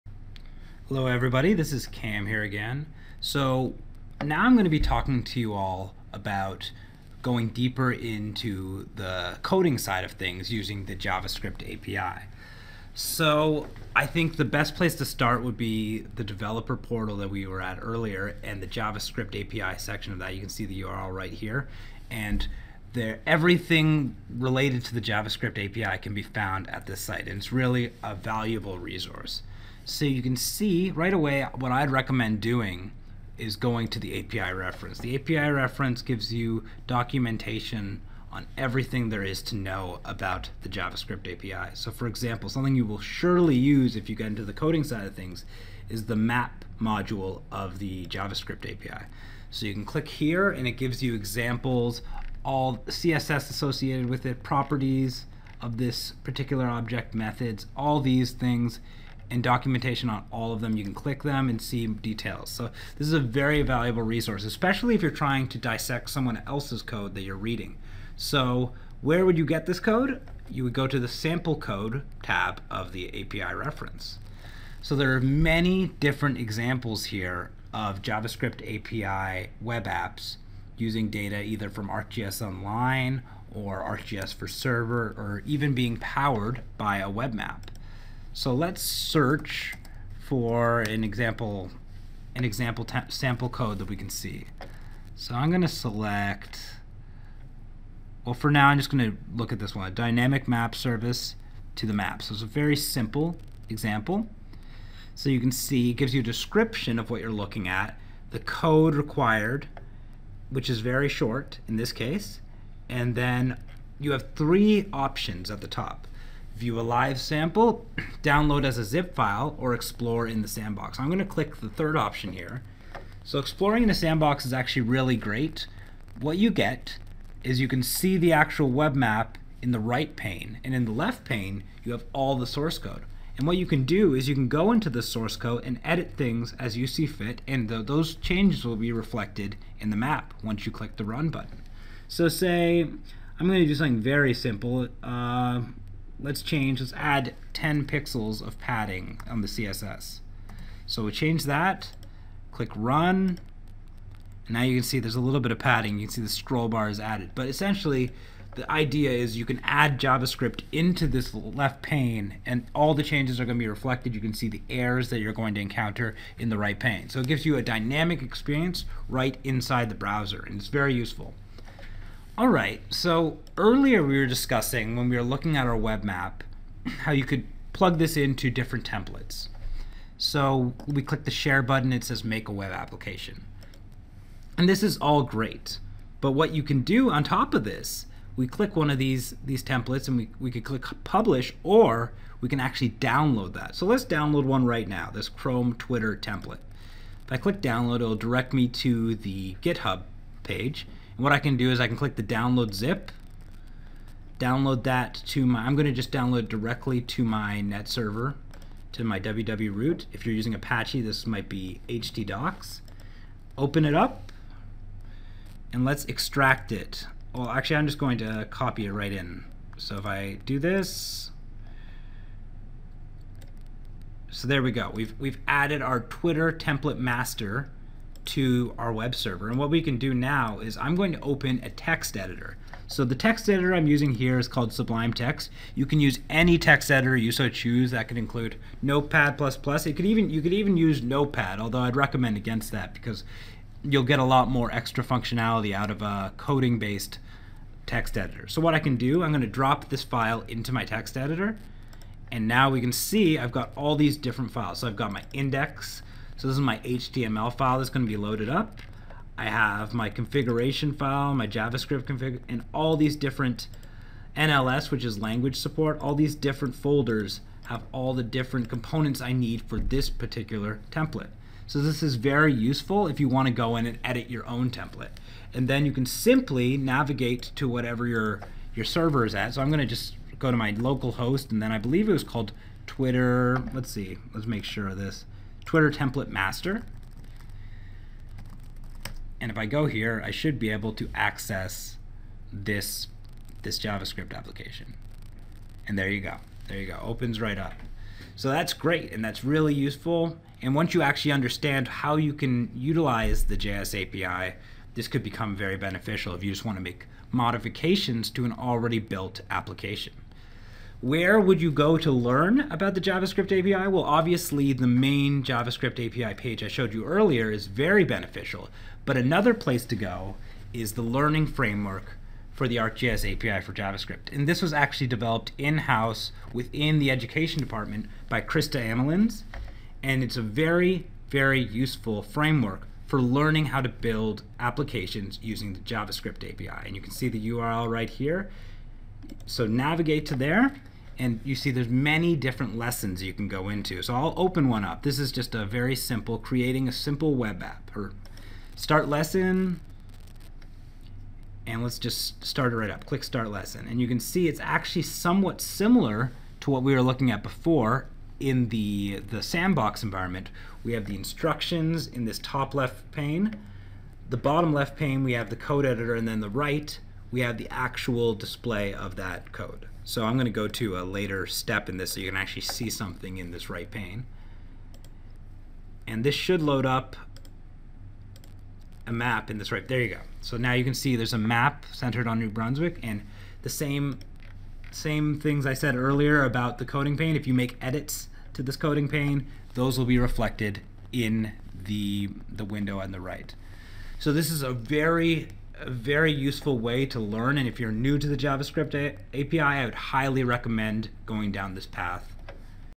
Getting started with the ArcGIS API for JavaScript
Cam demonstrates several useful resources and tools that you can use to help you hit the ground running with the ArcGIS JS API.